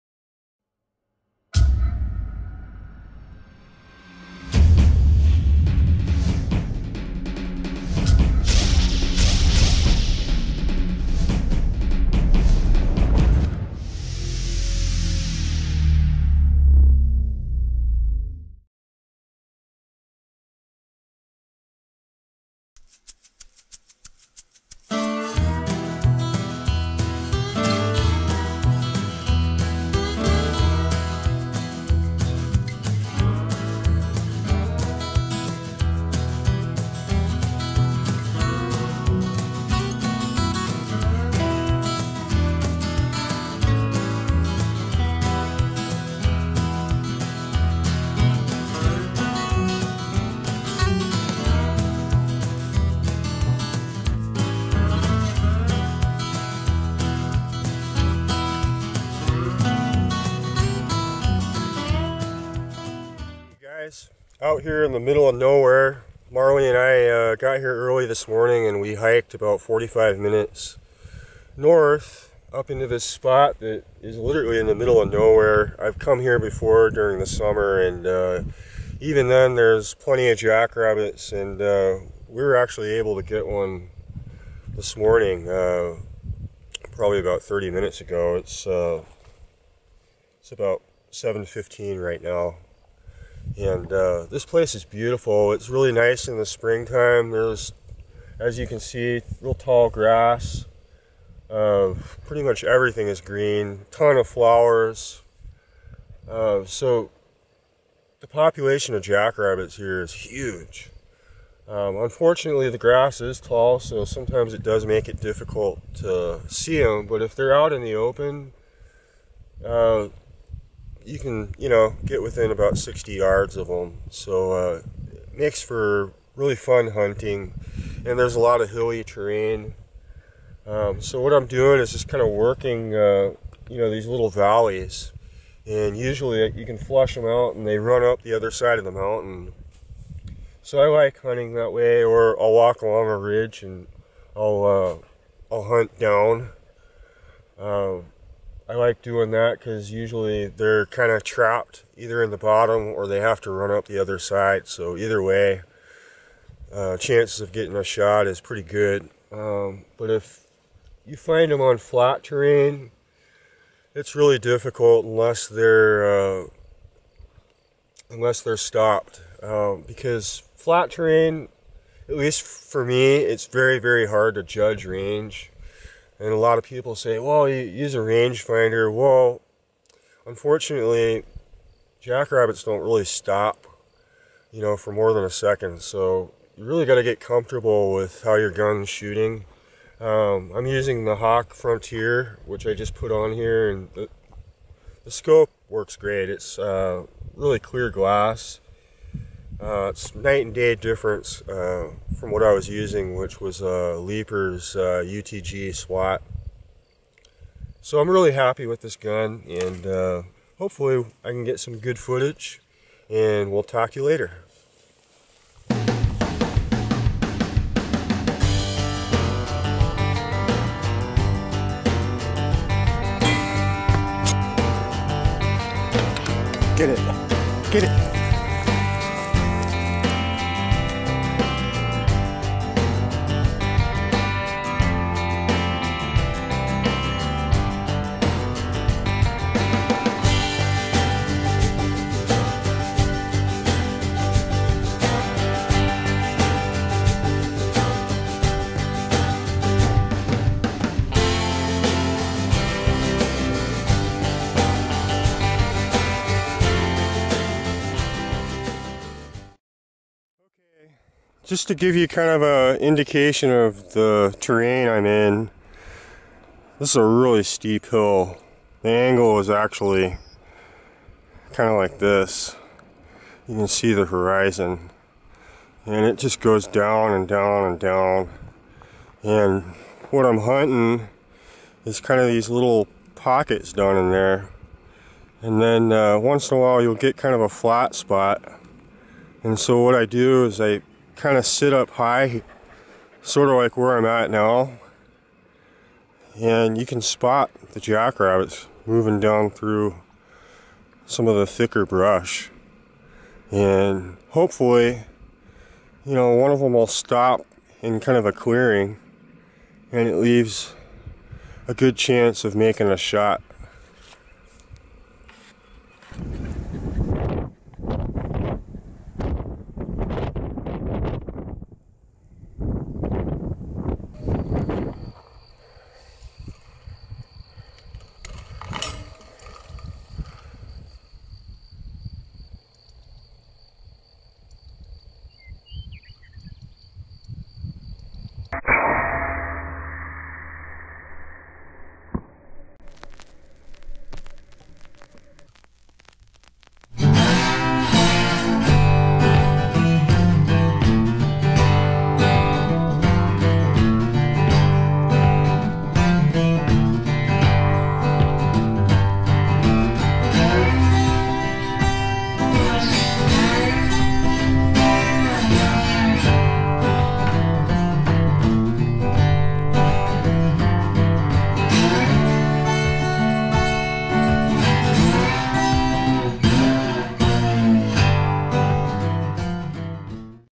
Airgun JACKRABBIT HUNTING with .30 EVOL
MountainsportAirguns takes you along for a short Jackrabbit Airgun hunt and offers a few tips on hunting them along with a 140 yard kill.